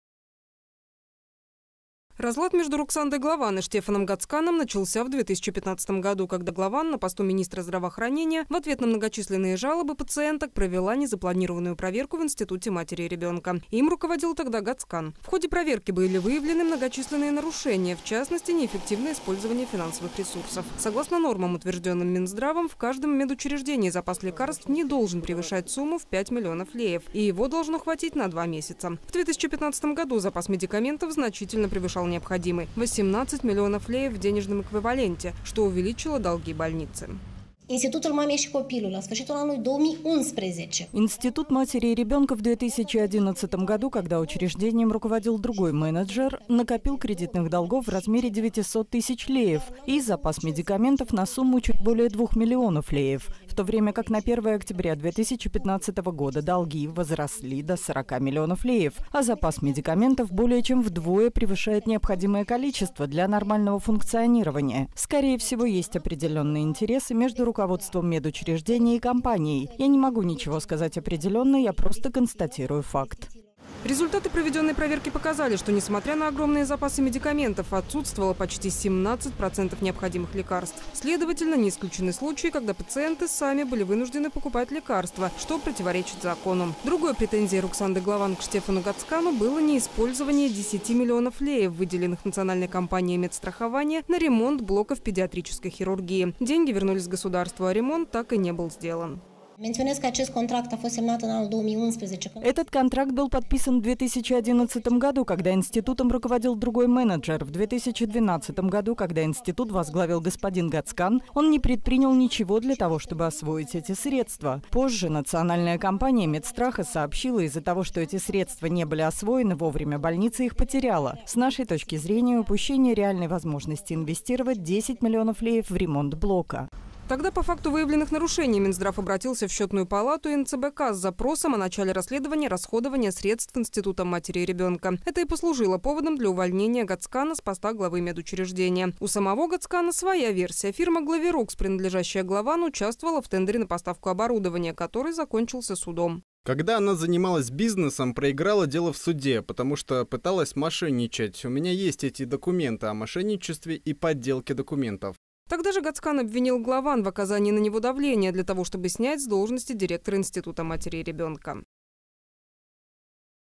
Уживутся ли 2 врача в одной партии?
За нецелевое использование средств Руксанда Главан уволила Штефана Гацкана – она тогда была министром здравоохранения, он руководил Институтом матери и ребенка. Гацкан обвинил Главан в мошенничестве и подделке документов. У новых однопартийцев давняя история взаимоотношений. Смогут ли ужиться 2 врача в одной партии, покажет время.

Разлад между Руксандой Главан и Штефаном Гацканом начался в 2015 году, когда Главан на посту министра здравоохранения в ответ на многочисленные жалобы пациенток провела незапланированную проверку в Институте матери и ребенка. Им руководил тогда Гацкан. В ходе проверки были выявлены многочисленные нарушения, в частности, неэффективное использование финансовых ресурсов. Согласно нормам, утвержденным Минздравом, в каждом медучреждении запас лекарств не должен превышать сумму 5 миллионов леев, и его должно хватить на 2 месяца.  В  2015 году запас медикаментов значительно превышал необходимый - в 18 миллионов леев в денежном эквиваленте, что увеличило долги больницы.

РУКСАНДА ГЛАВАН – МИНИСТР ЗДРАВООХРАНЕНИЯ (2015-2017)

«Институт матери и ребенка в 2011 году, когда учреждением руководил другой менеджер, накопил кредитных долгов в размере 900 тысяч леев и запас медикаментов на сумму чуть более 2 миллионов леев. В то время, как на 1 октября 2015 года долги возросли до 40 миллионов леев, а запас медикаментов более чем вдвое превышает необходимое количество для нормального функционирования. Скорее всего, есть определенные интересы между руководством медучреждения и компанией. Я не могу ничего сказать определенно, я просто констатирую факт».

Результаты проведенной проверки показали, что, несмотря на огромные запасы медикаментов, отсутствовало почти 17% необходимых лекарств. Следовательно, не исключены случаи, когда пациенты сами были вынуждены покупать лекарства, что противоречит закону. Другой претензией Руксанды Главан к Штефану Гацкану было неиспользование 10 миллионов леев, выделенных Национальной кампанией медстрахования на ремонт блоков педиатрической хирургии. Деньги вернулись государству, а ремонт так и не был сделан.

«Этот контракт был подписан в 2011 году, когда Институтом руководил другой менеджер. В 2012 году, когда институт возглавил г-н Гацкан, он не предпринял ничего для того, чтобы освоить эти средства. Позже, Национальная кампания медстраха сообщила, из-за того, что эти средства не были освоены вовремя, больница их потеряла. С нашей точки зрения, упущение реальной возможности инвестировать 10 миллионов леев в ремонт блока».

Тогда по факту выявленных нарушений Минздрав обратился в Счетную палату и НЦБК с запросом о начале расследования расходования средств Институтом матери и ребёнка. Это и послужило поводом для увольнения Гацкана с поста главы медучреждения. У самого Гацкана своя версия  - фирма «Glavirux», принадлежащая Главан, участвовала в тендере на поставку оборудования, который закончился судом.

ШТЕФАН ГАЦКАН – ГЛАВА ИНСТИТУТА МАТЕРИ И РЕБЕНКА (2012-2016)
«Когда она занималась бизнесом, проиграла дело в суде, потому что пыталась мошенничать. У меня есть эти документы о мошенничестве и подделке документов».

Тогда же Гацкан обвинил Главан в оказании на него давления, для того чтобы снять с должности директора Института матери и ребёнка.